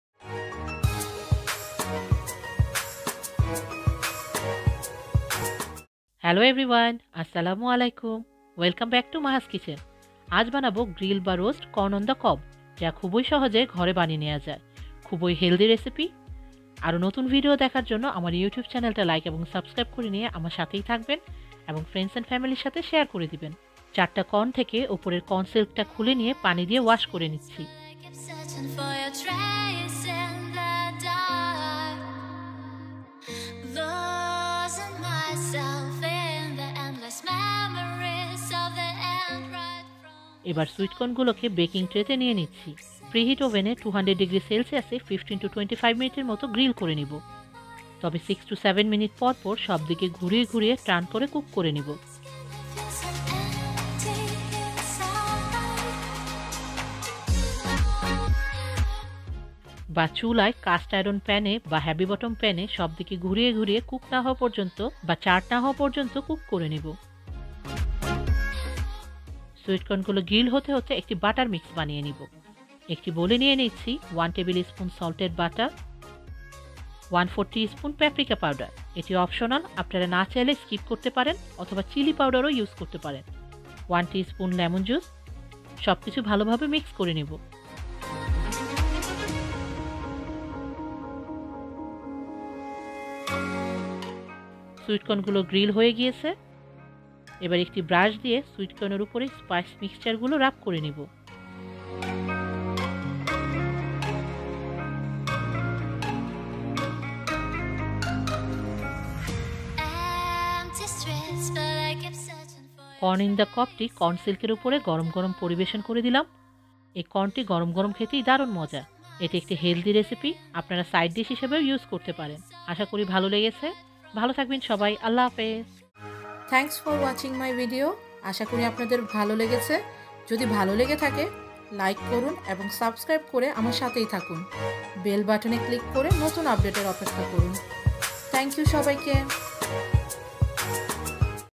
eng) Quick Corn On The Cob 🌽 | Pan & Oven | Grilled/Roasted Corn Recipe | Delicious & Buttery Corn
-English CC available, if not automatically check the settings icon in the video. It's usually available 30 mins after posting-

Hi everyone, do you love buttery, warm and delicious corn? With this corn and the cob recipe, you can tuck into some tasty corn along with your barbecue or meal. I hope you like the video!

INGREDIENTS:

4 sweetcorns
1 tablespoon of salted
1/4 teaspoon of paprika powder/chilli powder
1 teaspoon of lemon juice

Maha's Kitchen is now on TikTok! Make sure to check it out! @mahaskitchen2020 Maha's Kitchen is also on Instagram! @mahaskitchen.2020